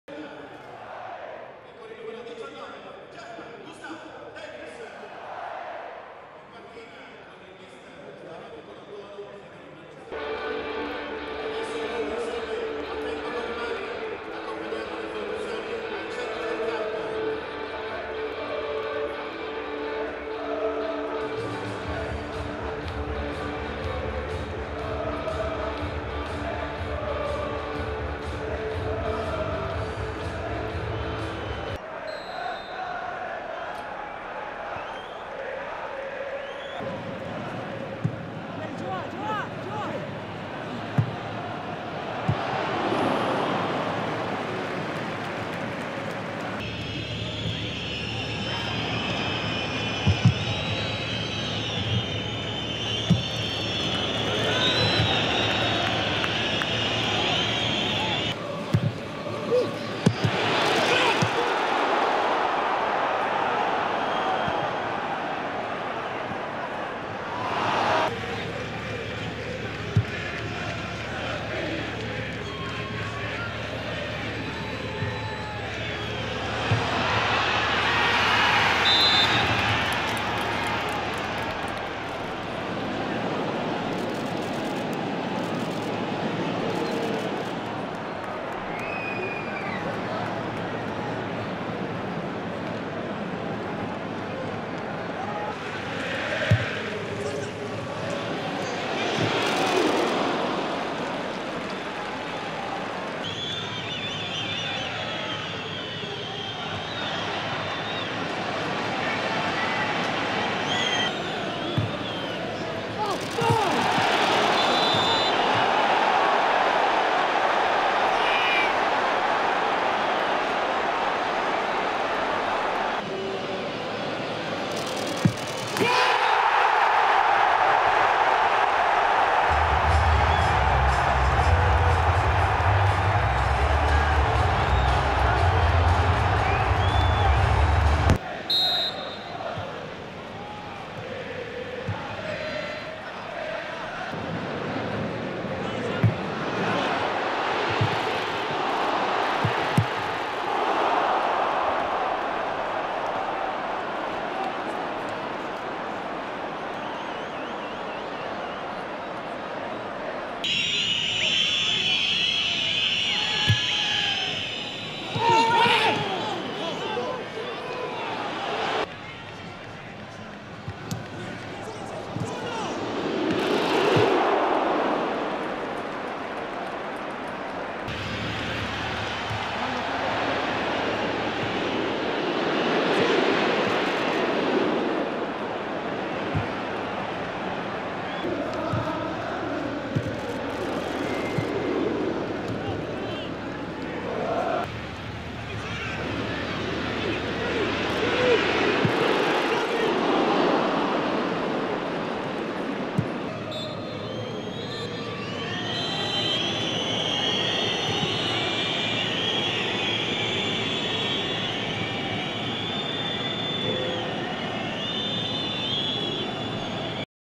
18ª giornata: Atalanta-Udinese 1-1
La sintesi della partita di serie A 2012/2013
Atalanta-Udinese 1-1 (Muriel 32' p.t., Denis 39' p.t.)